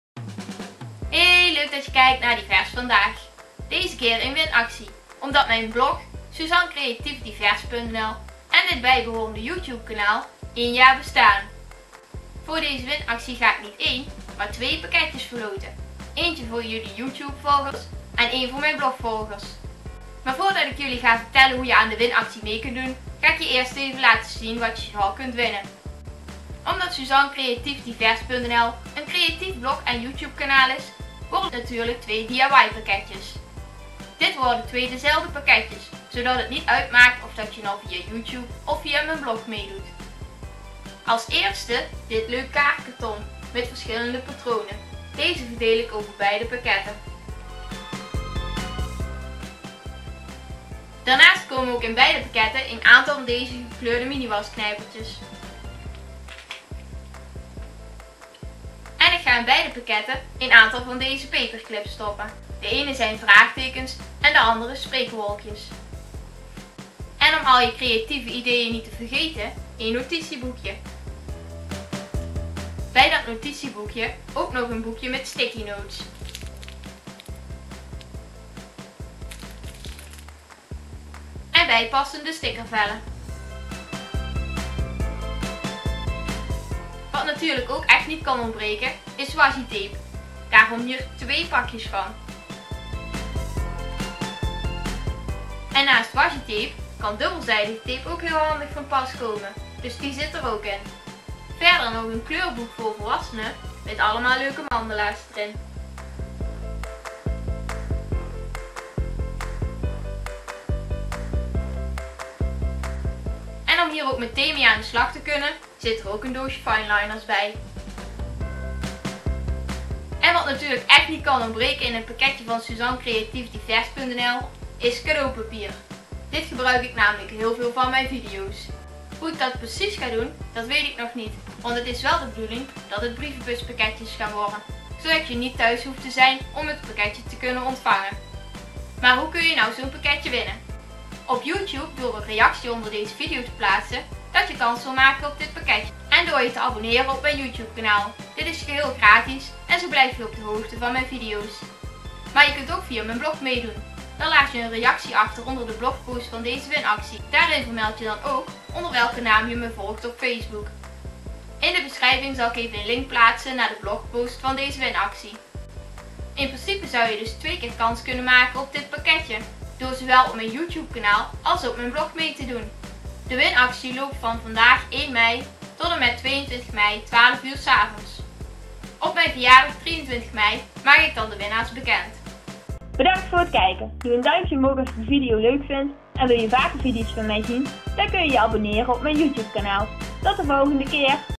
Divers vandaag: Winactie! SuzanCreatiefDivers.nl bestaat 1 jaar! (GESLOTEN)
Dus als jullie mij een mailtje willen sturen met als onderwerp "1 jaar winactie" naar en waarin je je naam en adres vermeld. Dan kan ik de pakketjes zo snel mogelijk naar jullie opsturen. Zodat jullie er lekker creatief mee aan de slag kunnen.

Hoe kun je mee doen!
Om aan deze winactie mee te doen moet je wel in Nederland wonen. (Sorry voor diegene die mij buiten Nederland volgen. Helaas worden de kosten anders namelijk te hoog.) Vervolgens zijn er 2 manieren om kans te maken op een pakketje.
✧ Via mijn blog door een reactie achter te laten onder deze blogpost dat je graag mee wilt doen. Vermeld hierin ook de naam waaronder je mij volgt op Facebook. (Door mij te volgen op Facebook blijf je op de hoogte van wat er op mijn blog gebeurt.)
✧ Via mijn YouTube-kanaal door een reactie onder de video achter te laten dat je kans wilt maken op 1 pakketje.

Wil je meer kans maken doe dan via mijn blog en YouTube-kanaal mee.

Hoelang kun je meedoen aan de winactie?
De winactie loopt van 1 mei tot en met 22 mei 23:59.
Vervolgens maak ik op mijn verjaardag 23 mei de winnaars bekend.

De inhoud van de pakketjes.
Het worden wel 2 dezelfde pakketjes, zodat het niet uitmaakt of je via mijn blog of via mijn Youtube-kanaal mee doet. (Het worden trouwens brievenbuspakketjes, zo hoef je namelijk niet thuis te zijn om ze te kunnen ontvangen.)

In beide pakketjes zitten de volgende spulletjes:
✧ Patronen karton
✧ Gekleurde miniwasknijpertjes
✧ Paperclips (Vraagteken en spreekwolken.)
✧ Cadeaupapier (2 soorten.)
✧ Notitieboekje
✧ Sticky notes boekje
✧ Stickervellen
✧ 2 pakjes washi tape
✧ Dubbelzijdige tape
✧ Kleuren voor volwassenen mandala boek
✧ Setje gekleurde fineliners